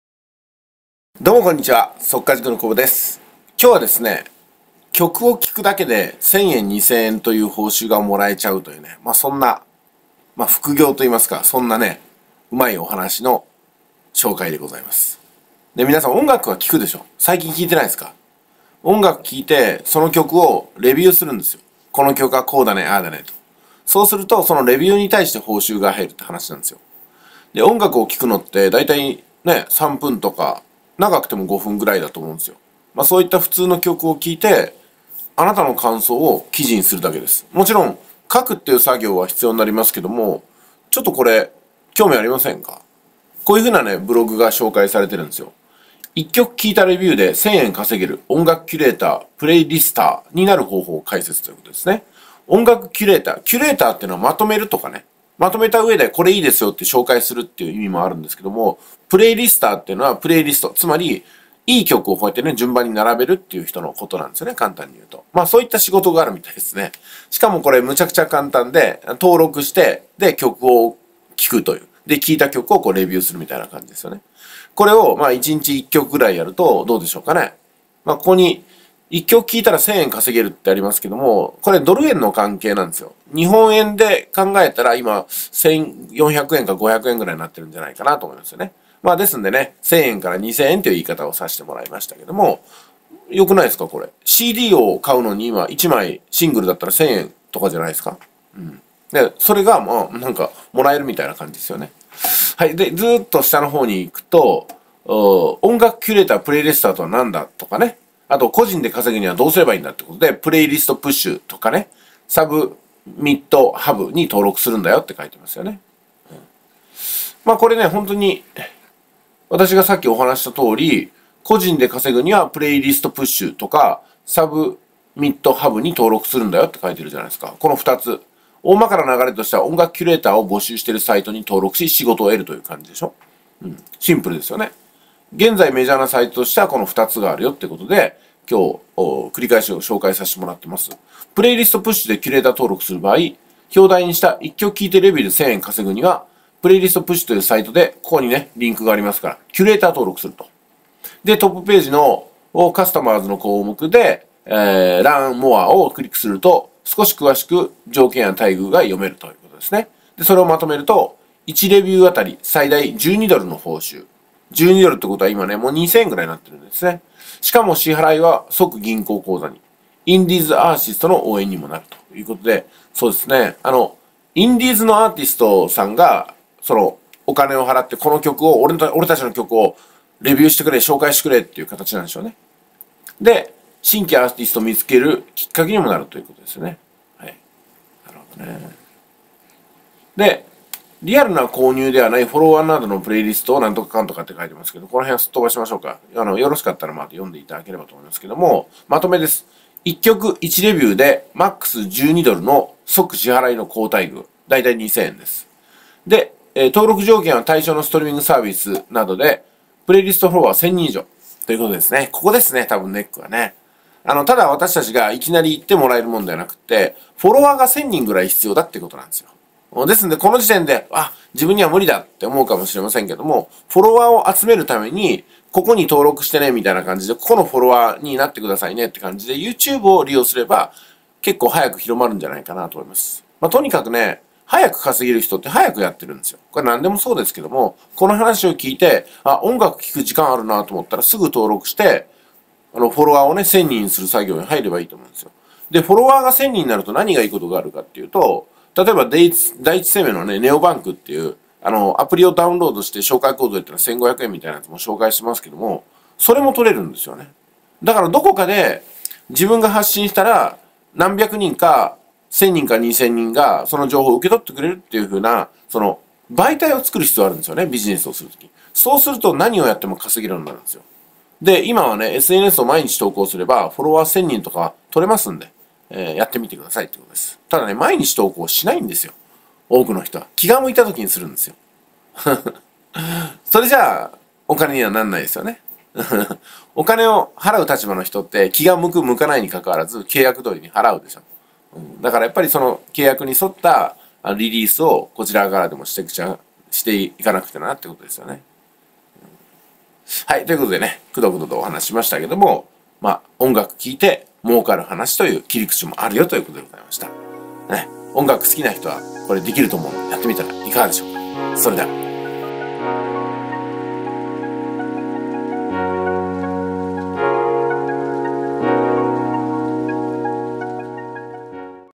【隠居TV】音楽聞いて「１曲2000円」報酬GET！ Playlist Push
音楽聞いて「１曲2000円」報酬GETな副業 Playlist　Push

「件名：速稼塾（あなたのお名前）」